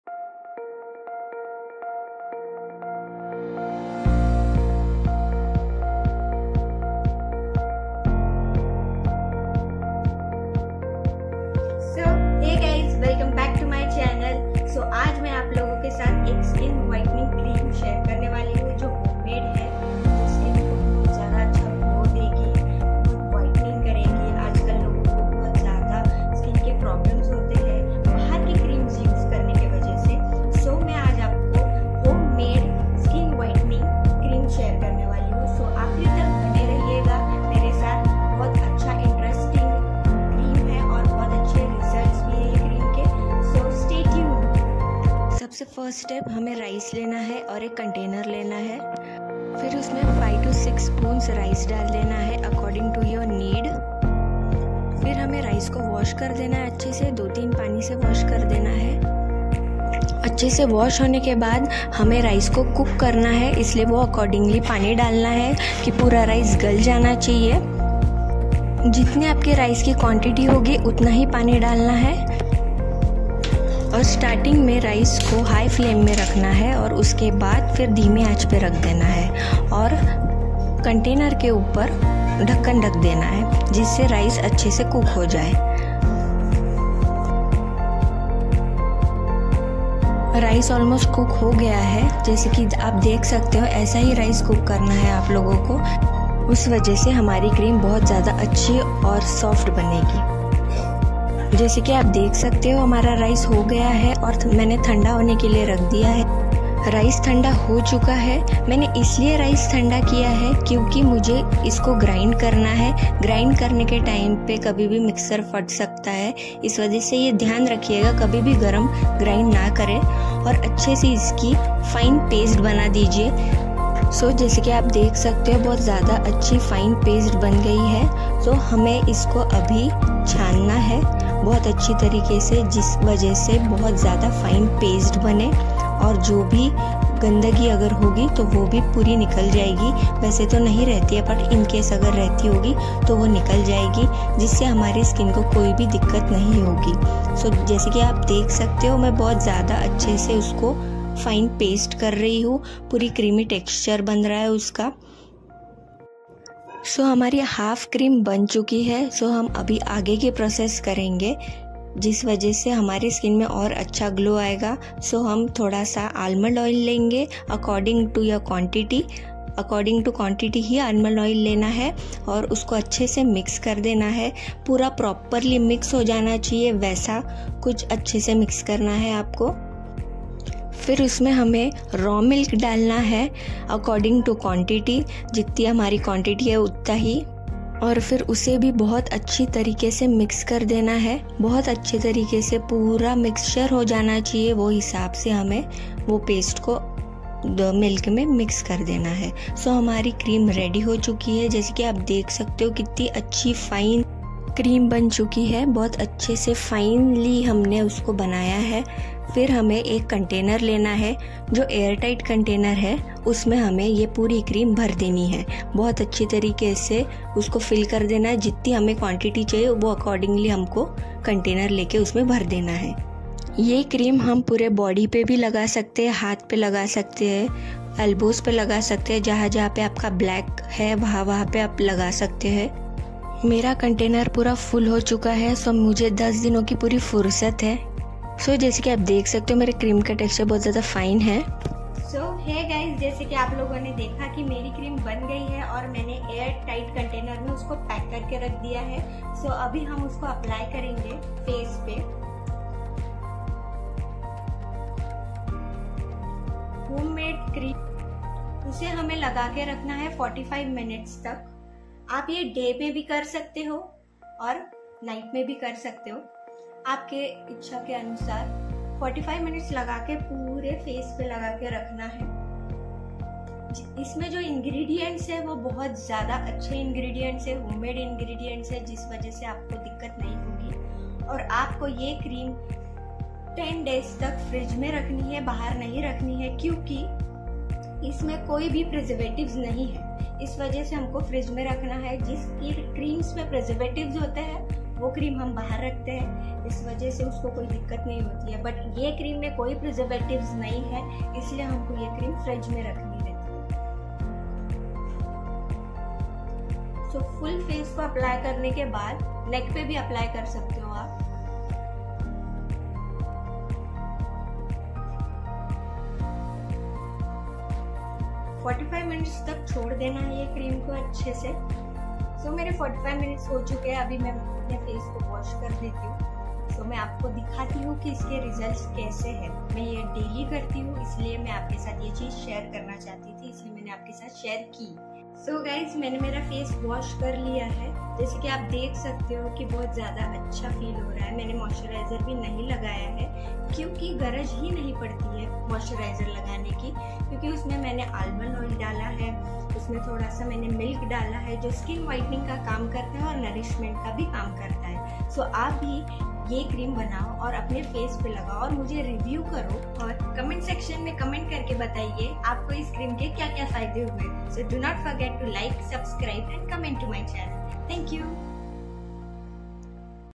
Home made Summer Cream For Glass skin
Recipe:- Take four spoon of rice wash it well two to three times place it in one container and add water and keep it for cooking, Cook it well. After rice is full cooked griend it well make a fine paste and add one tea spoon of almond oil and row milk mix it well and pore full mixture in one airtight container.